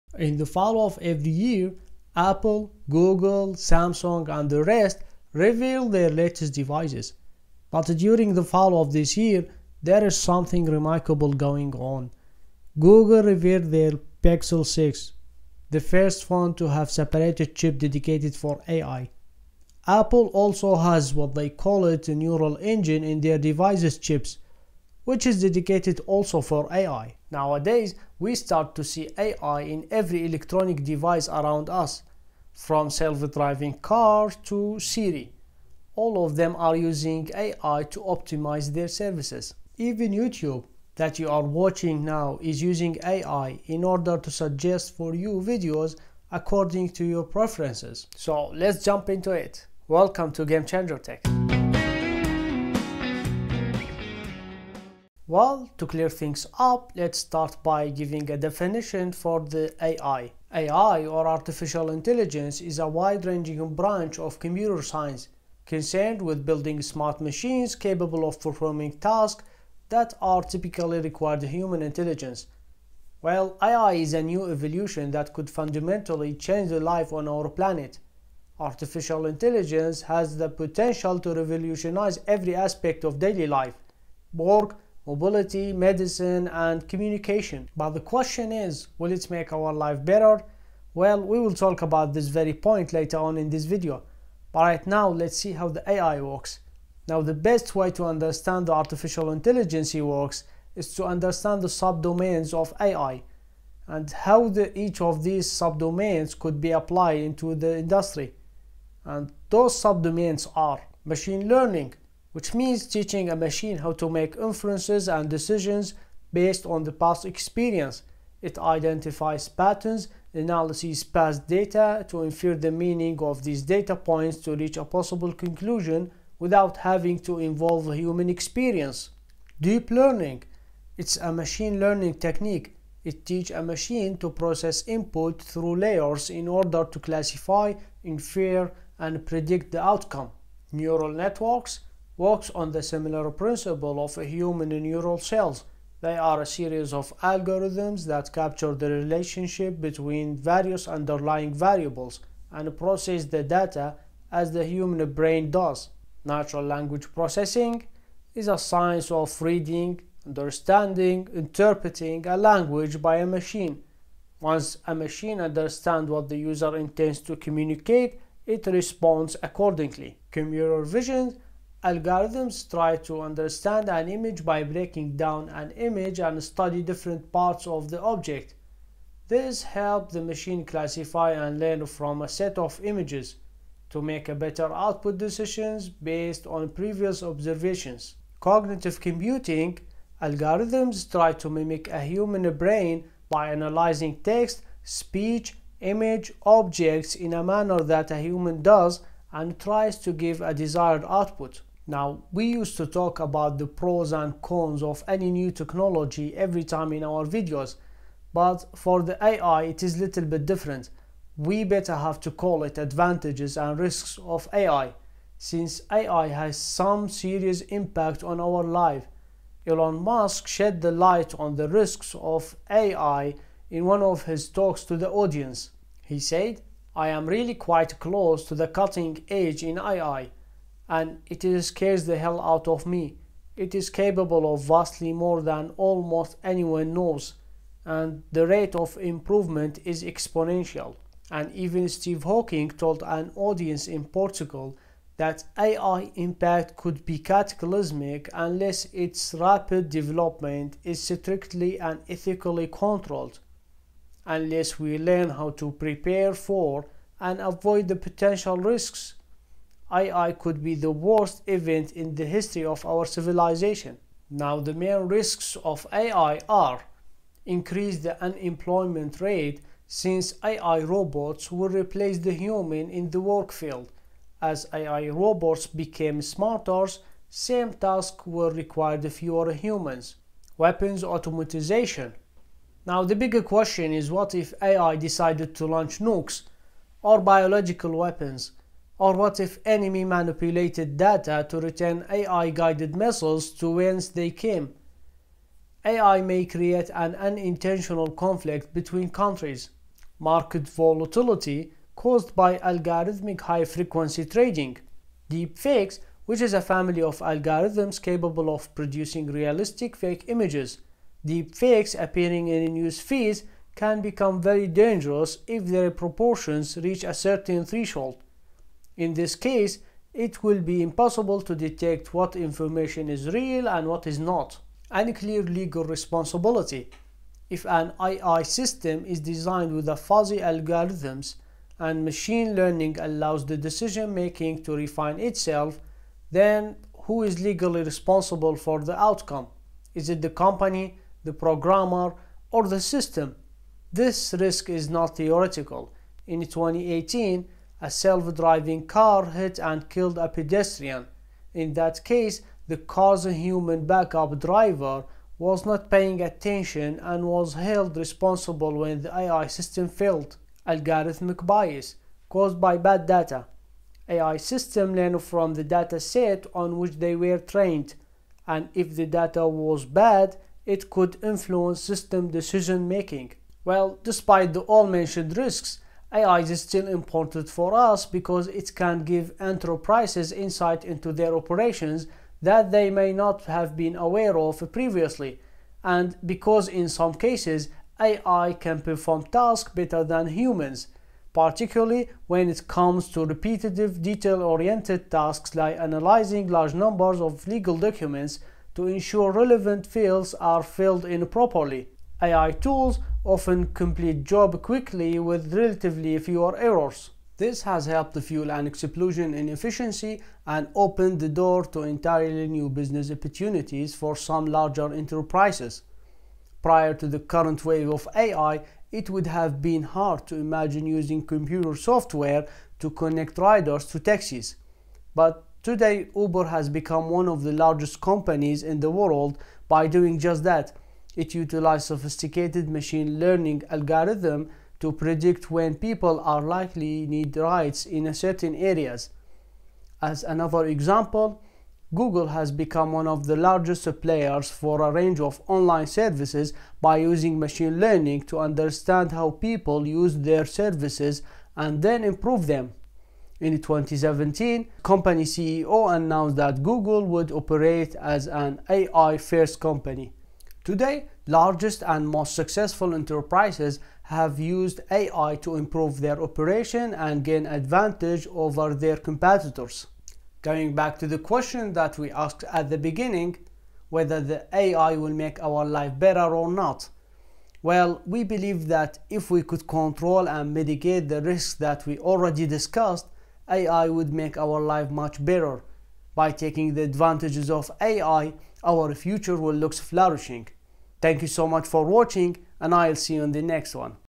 WHAT IS AI , ARTIFICIAL INTELLIGENCE , ADV. & RISK
Nowadays we start to see AI almost in every electronic device around Us , from self-driving car to Siri , all of them are using AI to optimize their services  .